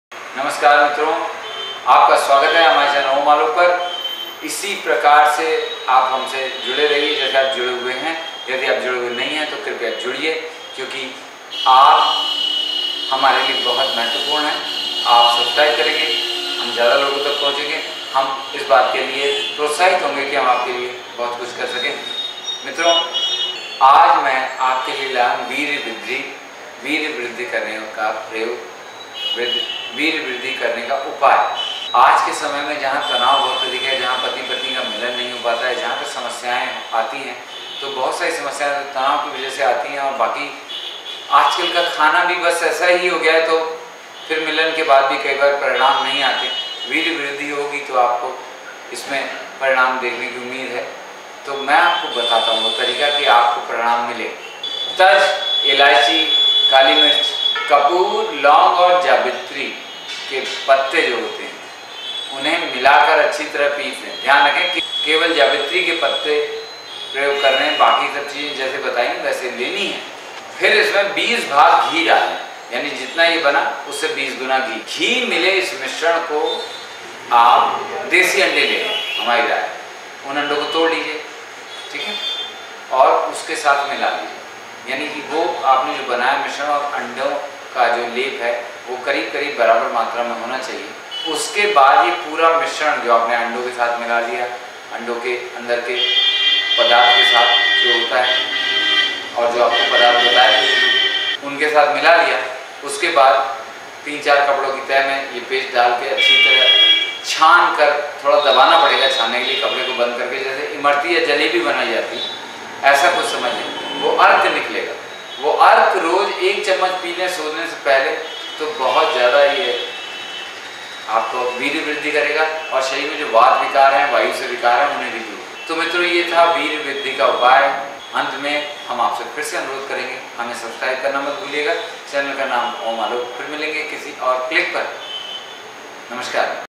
वीर्य वृद्धि का प्रभावशाली उपाय
संतान सुख न प्राप्त होने का कारण कभी कभी वीर्य की मात्रा काफी कम होना भी होता है. वीर्य कम होने से वीर्य की गुणवत्ता भी कम हो जाती है. इसका परिणाम ये होता है कि संतान होती है तो स्वस्थ नहीं होती. चौथा इसका प्रभाव ये होता है कि सम्भोग का आनंद काफी कम हो जाता है.
इन सभी दुष्परिणामों से बचना है तो कृपया ये वीडियो देखें.